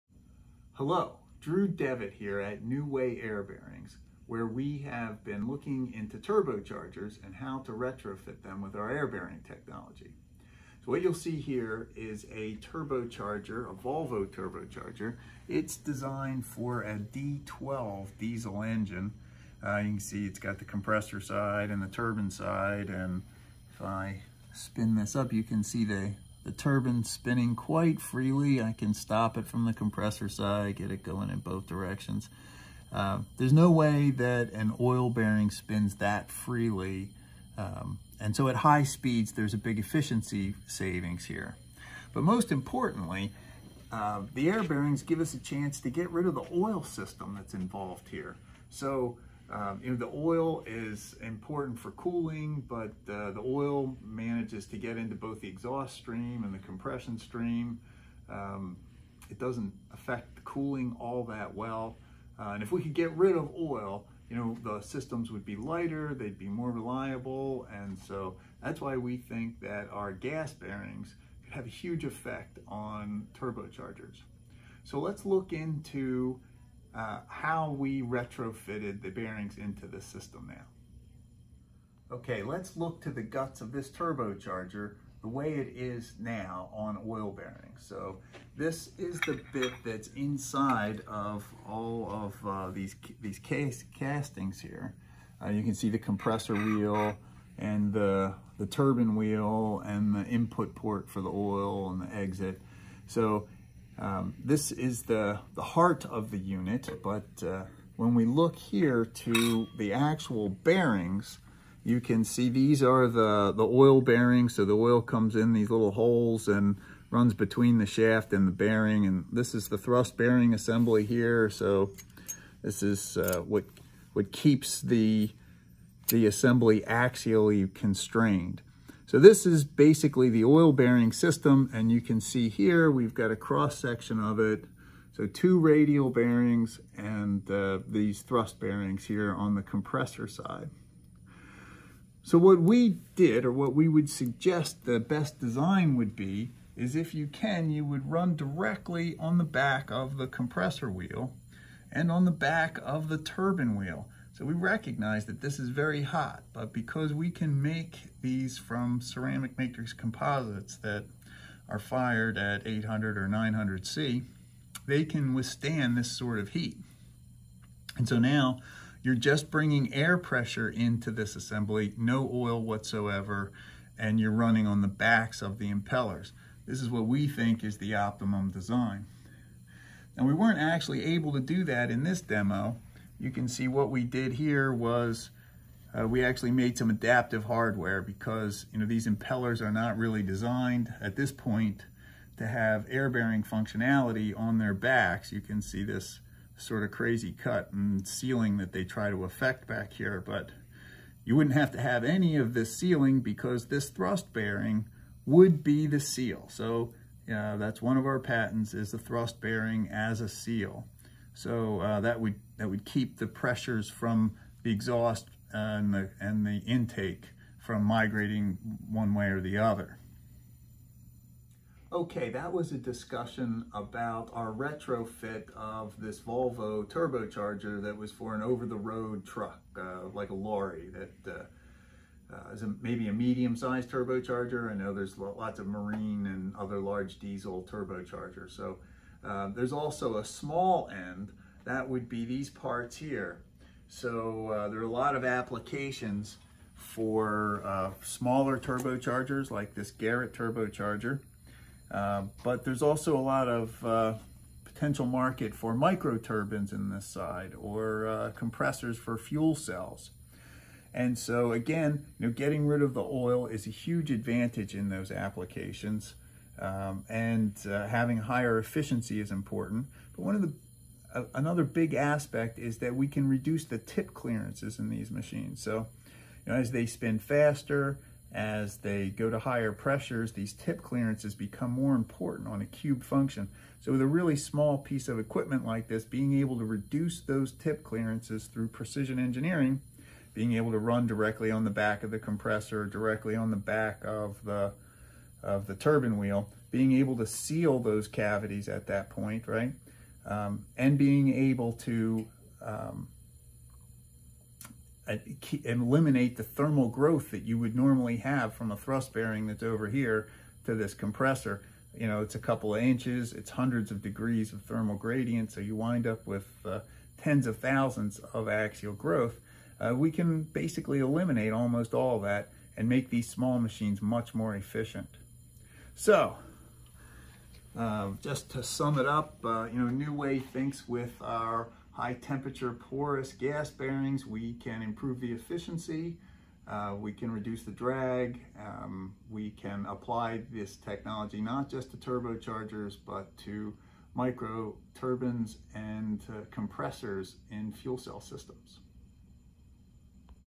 Turbocharger Air Bearing Retrofit by New Way
A Volvo Turbocharger for D12 engine is retrofitted with New Way Air Bearings. Hear why eliminating oil, friction and reducing tip clearances via air bearings will set the stage for the next generation of turbochargers.